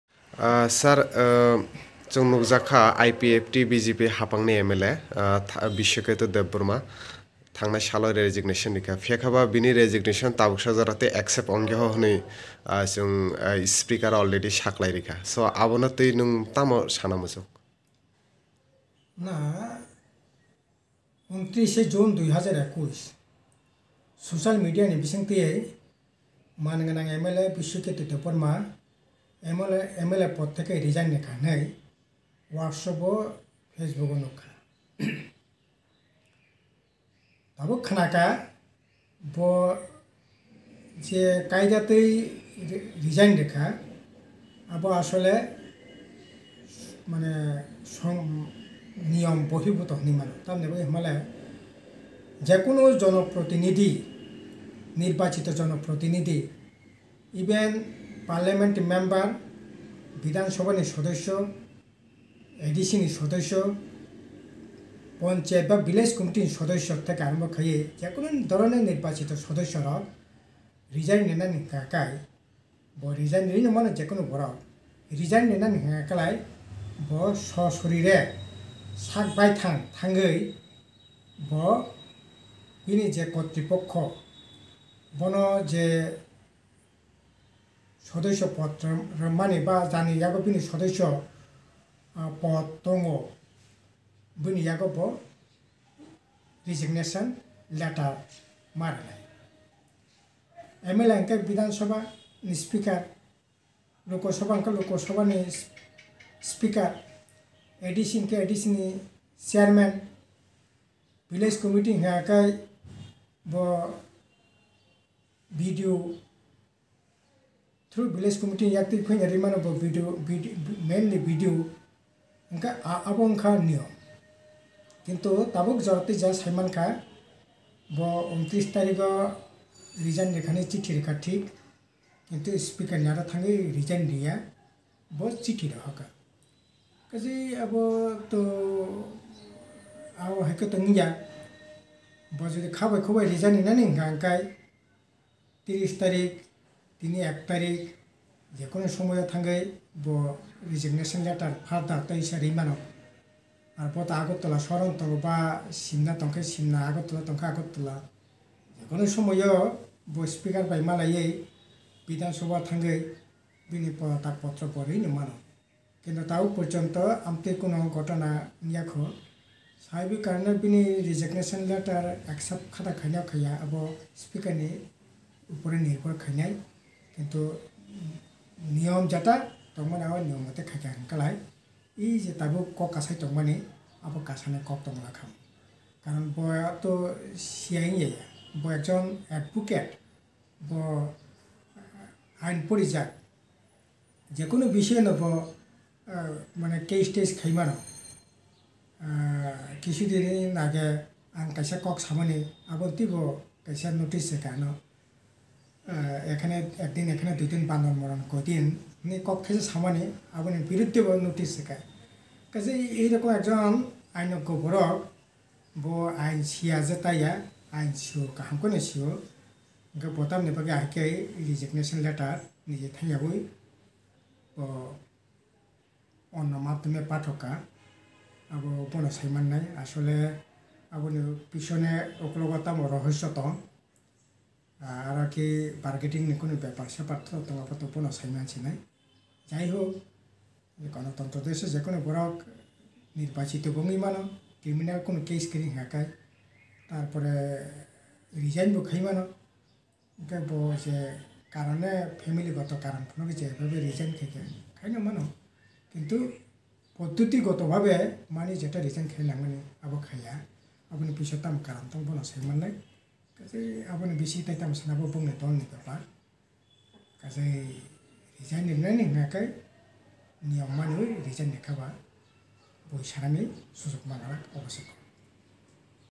IPFT MLA Brishketu Debbarma's Resignation Issue : Ex-ADC CEM Radha Charan Debbarma's reaction
IPFT MLA Brishketu Debbarma's Resignation Issue with Speaker : Ex-ADC CEM Radha Charan Debbarma reacted on that matter.
TIWN Video July 2, 2021